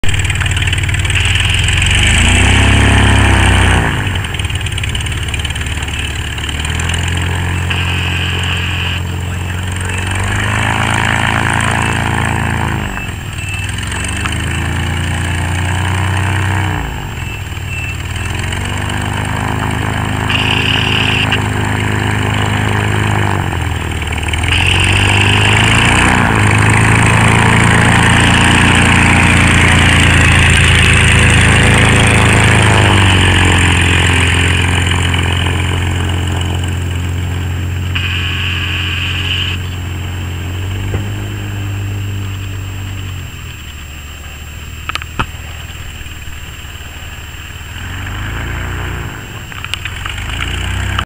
Corvair-engined Dominator Gyroplane on Alvord dry lakebed in SE Oregon
On the Alvord dry lake bed, July 2015. Jay taxiing around with his gyroplane that "runs like a sewing machine" (his words) powered by 6 cylinder,100 hp, GM Corvair engine.